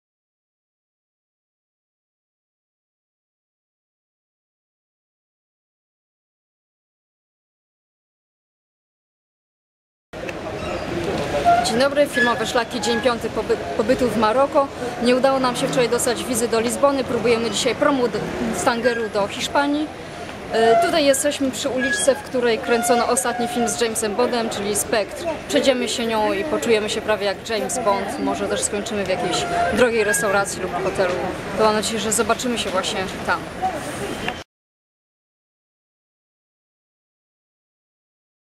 Tanger - Filmowe Szlaki w Maroko
Dzień 5. Tanger. U styku Afryki i Europy. Lokacja ze "Spectre"